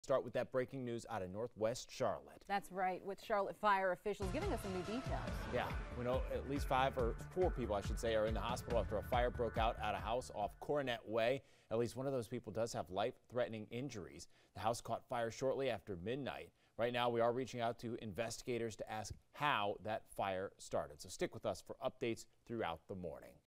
4 hurt in northwest Charlotte house fire; latest in string of significant fires
The fire broke out on Coronet Way early Friday morning.